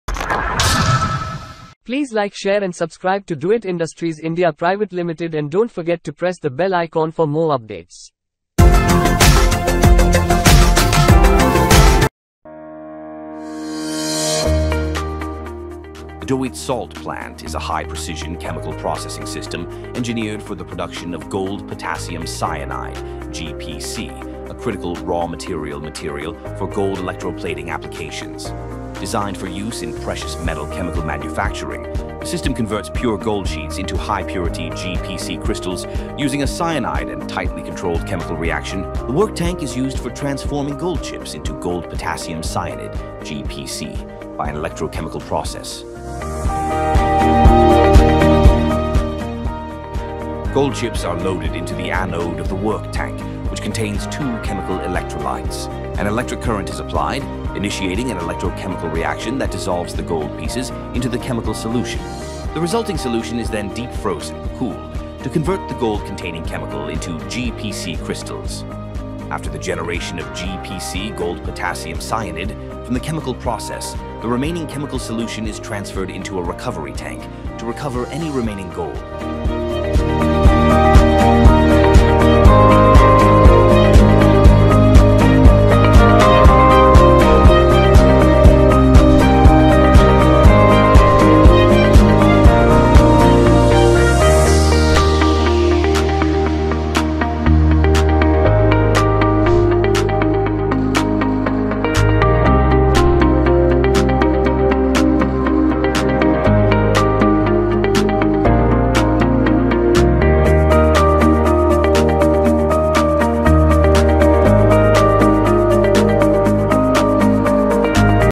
Doit Salt Plant | High-Precision GPC Production System | Doit Industries Limited
The Doit Salt Plant is a high-precision chemical  processing system engineered for the production of Gold Potassium Cyanide (GPC)— a critical raw material for gold electroplating  applications. Designed for use in precious metal chemical manufacturing, the system  converts pure gold sheets into high-purity GPC  crystals using a cyanide and tightly controlled  chemical reaction.